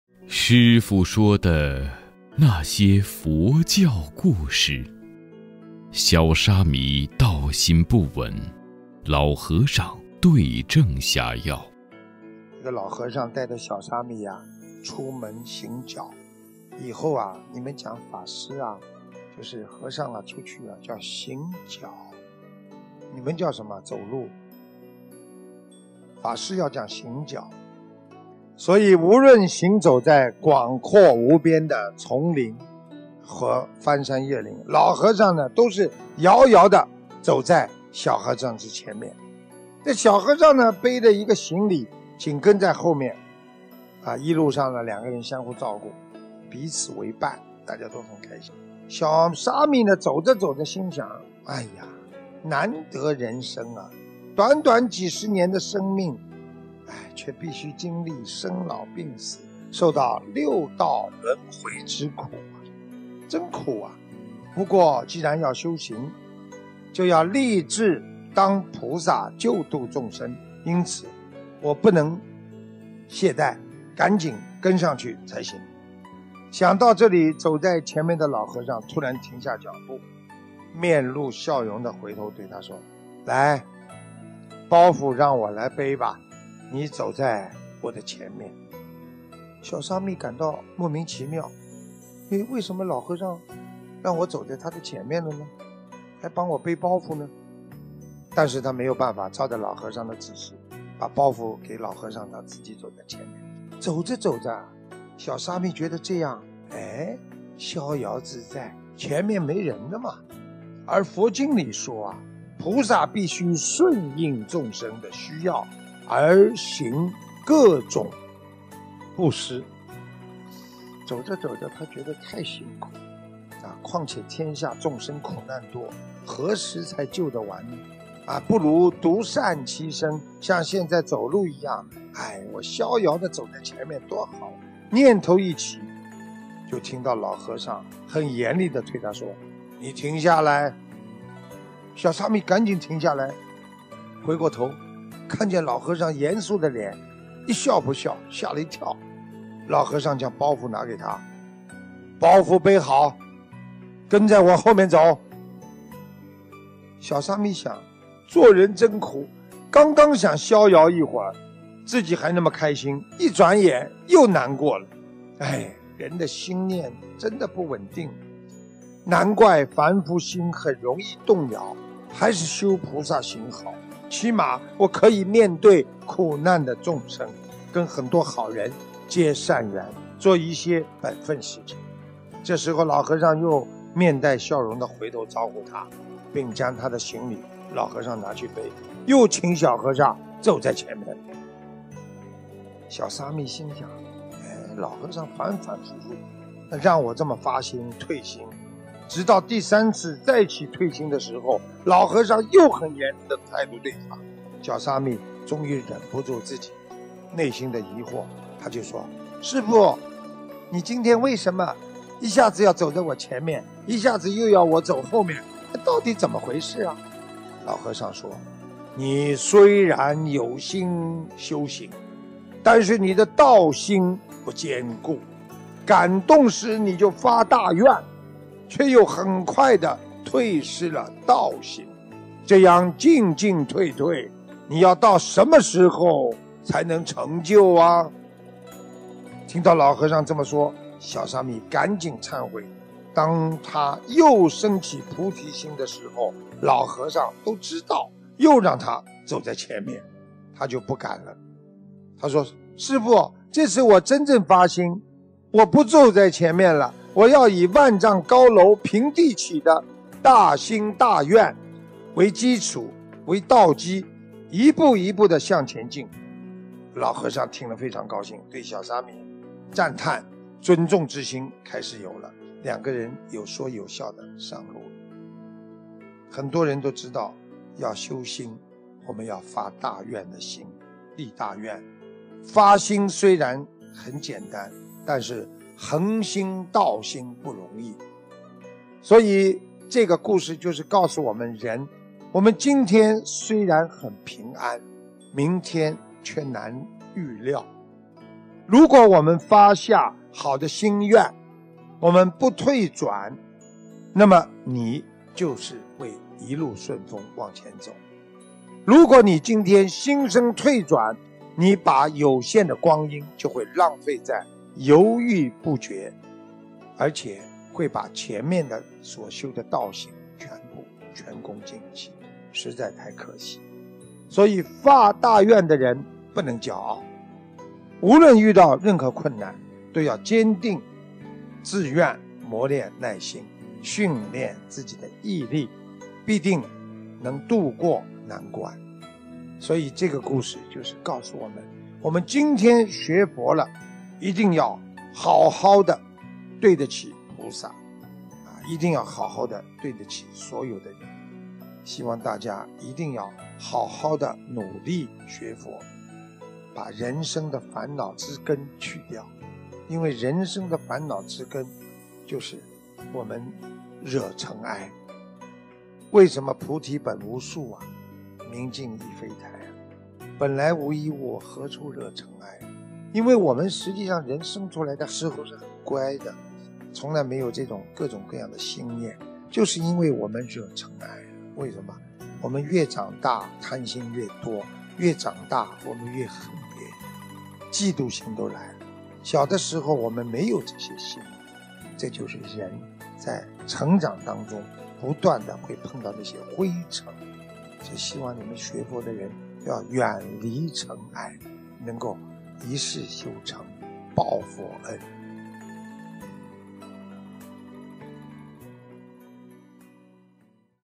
师父说的那些佛教故事【小沙弥道心不稳 老和尚对症下药】
11 CHANG CHARN ROAD #05-01, SHRIRO HOUSE, SINGAPORE 159640.

【小沙弥道心不稳 老和尚对症下药】
一个老和尚带着小沙弥出门行脚，无论行走在广阔无边的丛林或翻山越岭，老和尚都是遥遥地走在小和尚的前面，小和尚背着一个行李紧跟在后面，一路上两人相互照顾，彼此为伴。

小沙弥走着走着，心想：“难得人身，短短几十年的生命，却必须经历生老病死，受到六道轮回之苦，真苦啊！不过，既然要修行，就要立志当菩萨救度众生；因此我不能懈怠，赶紧跟上去才行！”
想到这里，走在前面的老和尚突然停下脚步，面露笑容地回头对他说：“来，包袱让我来背吧，你走在我的前面。”小沙弥感到莫名其妙，“为什么老和尚让我走在他的前面，还帮我背包袱呢？”但是他没有办法，照着老和尚的指示，把包袱给老和尚，他自己走在前面。

走着走着，小沙弥觉得这样逍遥自在！而佛经里说，菩萨必须顺应众生的需要而行各种布施，他觉得：“太辛苦了！况且天下众生苦难多，何时才救得完呢？不如独善其身，像现在走路一样，我逍遥地走在前面，多好啊！”念头一起，就听到老和尚很严厉地对他说：“你停下来！”小沙弥赶紧停下来，回过头看见老和尚严肃的脸，吓了一跳！老和尚将包袱拿给他：“包袱背好，跟在我后面走！”
小沙弥想：“做人真苦！刚刚想逍遥一会儿，自己还那么开心，一转眼又难过了，人的心念真的不稳定啊！难怪，凡夫心很容易动摇，还是修菩萨行好，起码我可以面对苦难的众生，跟很多好人结善缘，做一些本分事情。”这时老和尚又面带笑容地回头招呼他，并将行李老和尚拿去背，又请小和尚走在前面。

小沙弥心想“老和尚反反复复让我这么发心、退心”，直到第三次再起退心的时候，老和尚又用很严厉的态度对他。小沙弥终于忍不住自己内心的疑惑，他说：“师父，您今天为什么一下子要我走在前面，一下子又要我走后面，到底是怎么一回事呢？”老和尚说：“你虽然有心修行，但是你道心不坚固：感动时就发大愿，却又很快退失了道心。这样进进退退，你要到什么时候才能成就啊？”
听到老和尚这么说，小沙弥赶紧忏悔，当他又生起菩提心的时候，老和尚又让他走在前面，他就不敢了，他说：“师父，这次我真正发心，我不走在前面了，我要以万丈高楼平地起的大心大愿为道基，一步一步地向前进。”老和尚听了非常高兴，对小沙弥的赞叹、尊重之心开始有了，两人有说有笑地上路了。

很多人都知道，要修心，我们要发大愿的心、立大愿。发心虽然很简单，但是恒心道心不容易。
我们今天虽然很平安，明天却难预料。如果发下好的心愿，不退转，就会一路顺风往前走；如果心生退转，把有限的光阴就会浪费在犹豫不决上，而且会使前面所修的道心全部前功尽弃，实在太可惜！所以，发大愿的人不能骄傲，无论遇到任何困难，都要坚定志愿、磨练耐心、训练自己的毅力，必定能度过难关。

这个故事告诉我们，我们今天学佛了，一定要好好地对得起菩萨，对得起所有的人。

希望大家一定要好好地努力学佛，把人生的烦恼之根去掉。人生的烦恼之根就是惹尘埃。菩提本无树，明镜亦非台，本来无一物，何处惹尘埃。人出生的时候都是很乖的，从来没有各种各样的心念，就是因为我们惹尘埃了，越长大，贪心越多；越长大，越恨别人，嫉妒心都来了，小的时候我们没有这些心的，这就是人在成长中不断会碰到灰尘。

希望你们学佛人要远离尘埃，能够一世修成报佛恩。